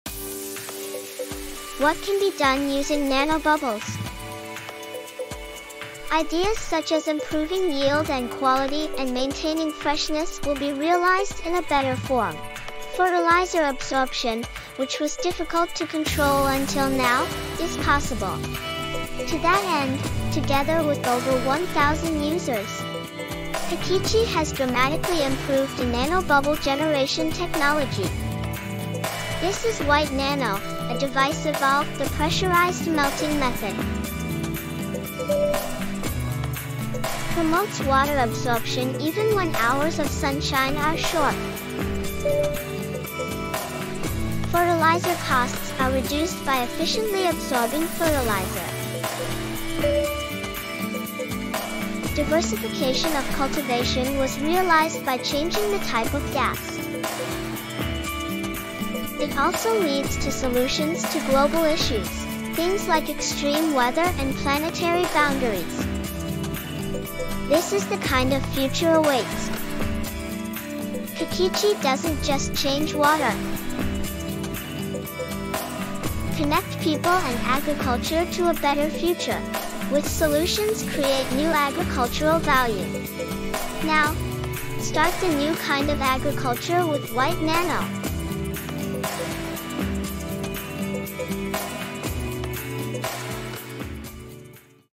The new nano bubble "White Nano" ~VALUE to WATER~ ENG ver.
Value to water , Change our future.

対象機：26030タイプ（流量30L/分）
計測日：2021/3/19
計測機：Nanosight LM10（Malvern社製）を用いて自社で計測
計測結果：3.2億個/ml （平均気泡径110nm）
（水質により発生個数は異なる場合があります）

「ナノバブル」「NANO BUBBLE」は